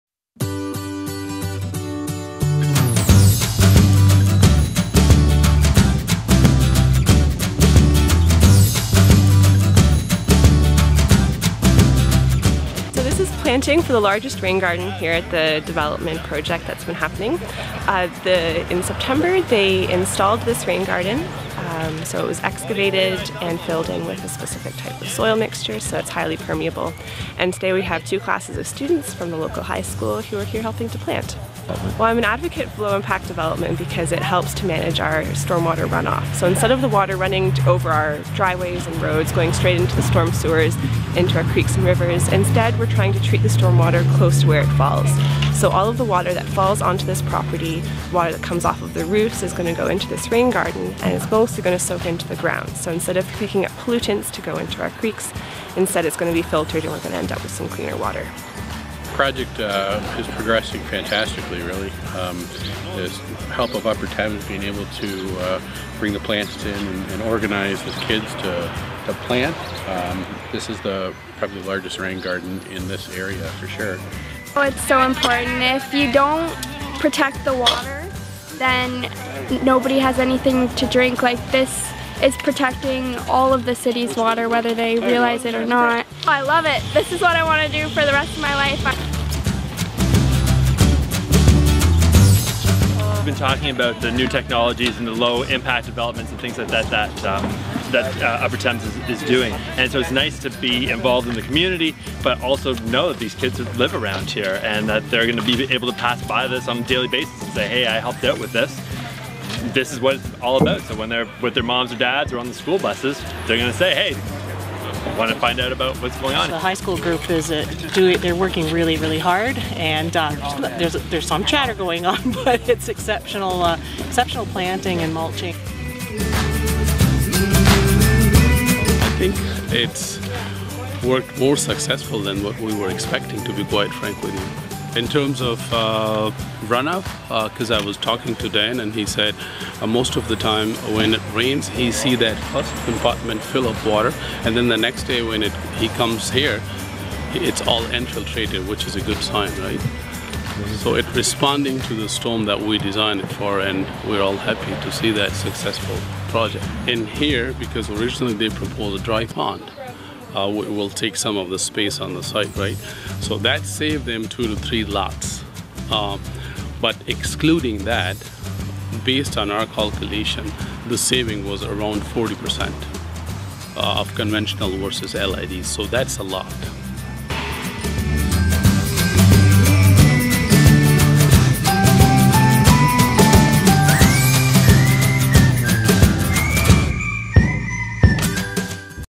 Low Impact Development Ingersoll Part 3
Low Impact Development provides solutions to infiltrate, filter, store and evaporate stormwater runoff while reducing site development costs for the developer.

The UTRCA’s project partners include Warren Sinclair Homes, the Town of Ingersoll, Dillon Consulting Ltd., Ingersoll District Secondary School, the Ontario Ministry of the Environment and Climate Change, and Environment Canada.